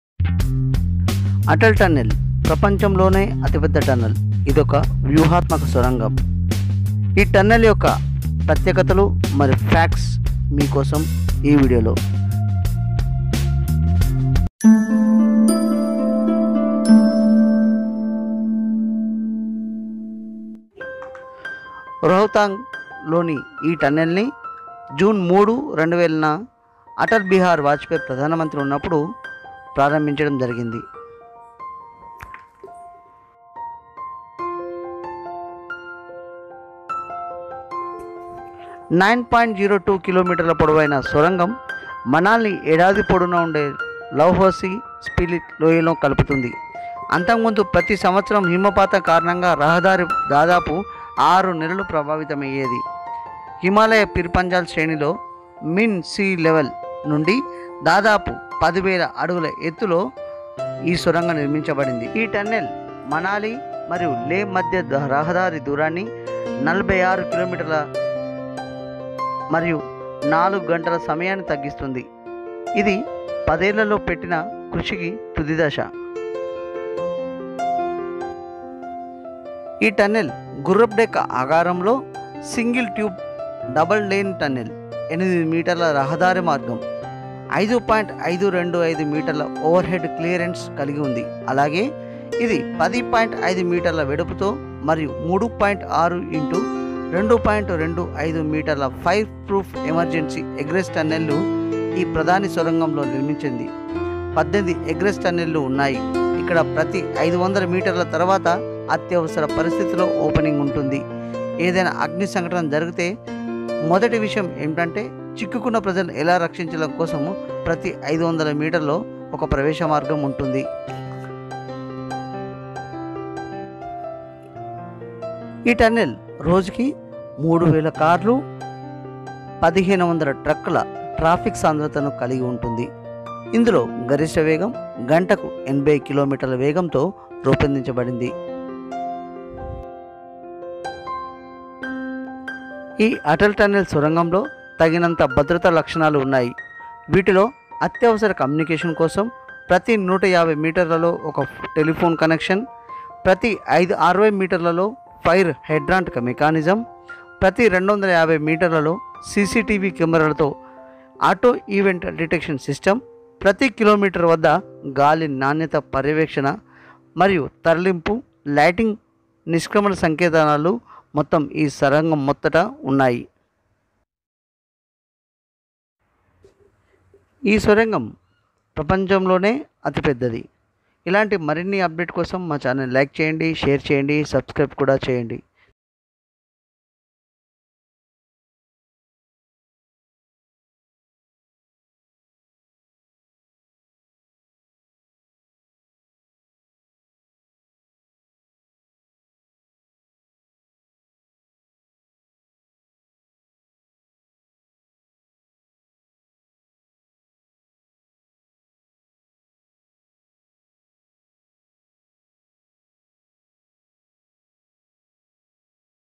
ప్రపంచంలోనే అతిపెద్ద టన్నెల్ (ATAL TUNNEL )గురించి ఇంట్రెస్టింగ్ ఫాక్ట్స్
ప్రపంచంలోనే అతి పెద్ద రోడ్డు టన్నెల్ అనిల్ గురించి ఇంట్రెస్ట్ ఫ్యాక్ట్స్ ఈ వీడియోలో మీకోసం...